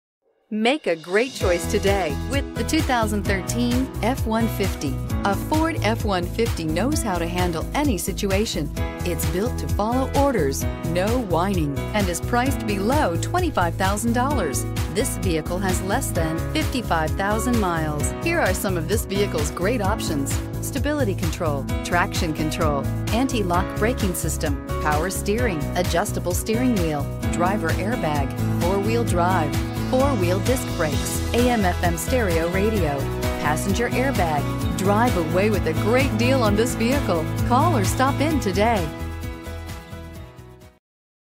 2013 Ford F-150 T4199A
2013 FORD F-150 Grafton, WV
Stock# T4199A

Toothman Ford
Us Route 50 West

Looking for the right car? Today could be your lucky day. With 51927 miles, this , 2013 Ford F-150 equipped with Automatic transmission could be yours. Request more information and set up a test drive right away.